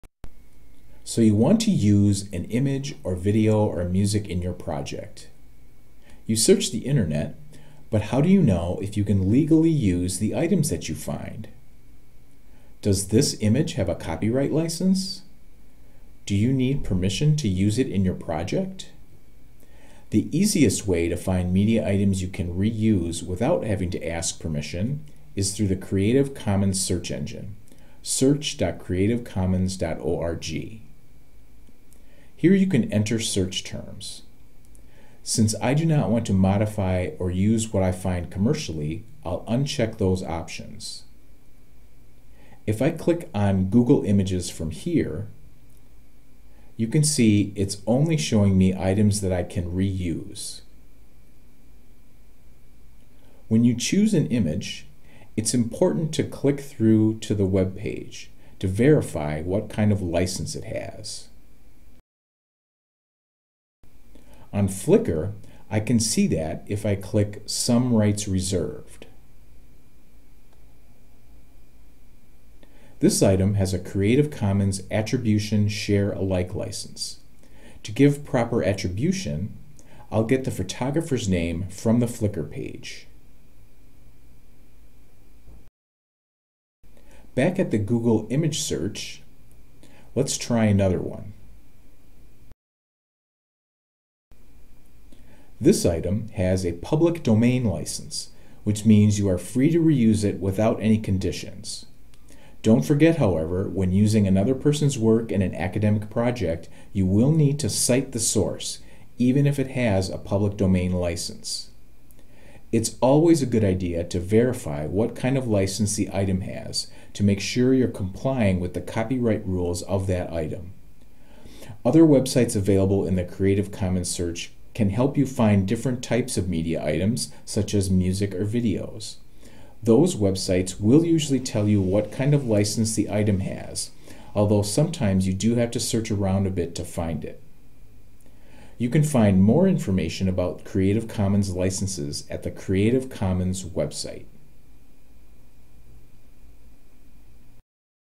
Find Creative Commons Items
How to find multimedia items with Creative Commons or Public Domain licenses, using the Creative Commons search.